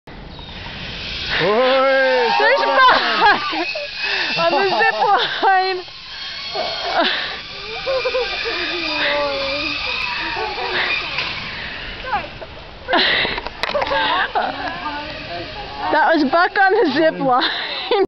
Vision Valley - Buck on the Flying Fox
Buck on the Flying Fox (aka: zip line) at Vision Valley